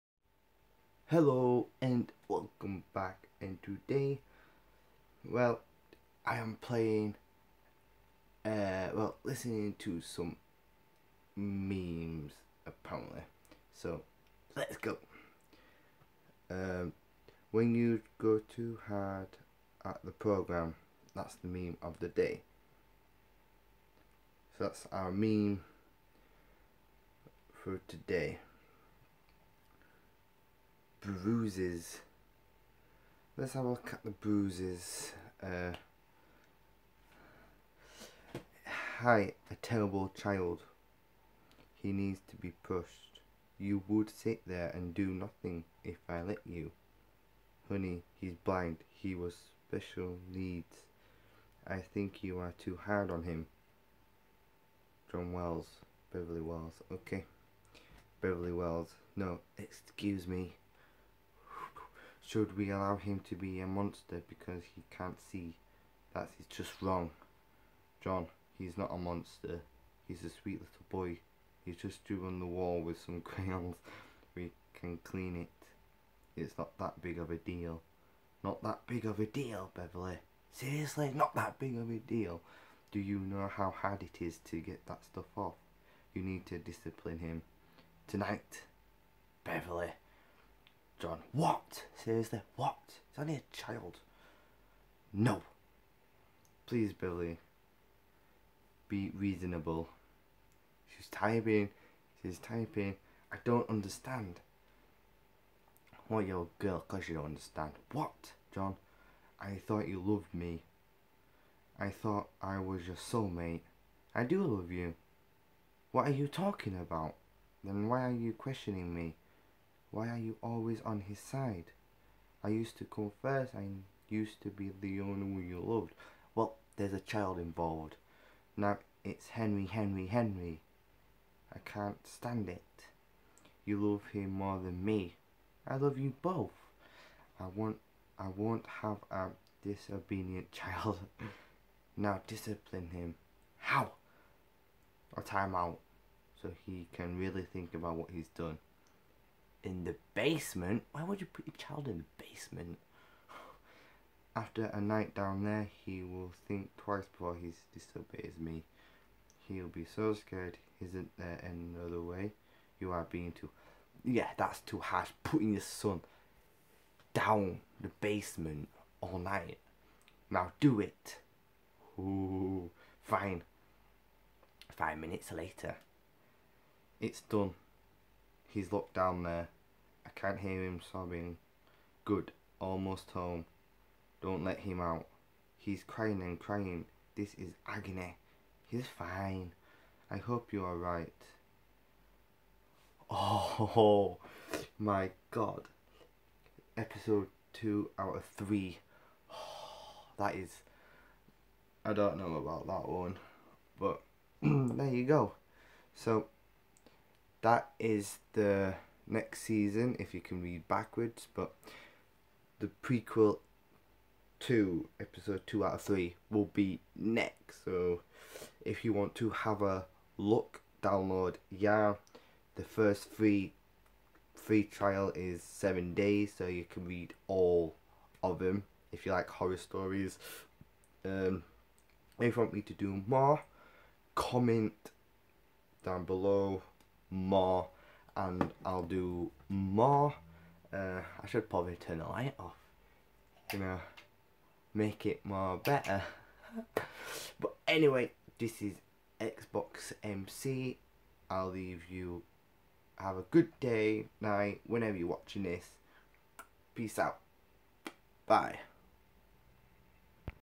Yarn: Bruises ep 1
Hello and welcome back  to another video and in this video I am reading "Bruises" there are 3 prequel.  The series are so good that you won't be able to sleep at night's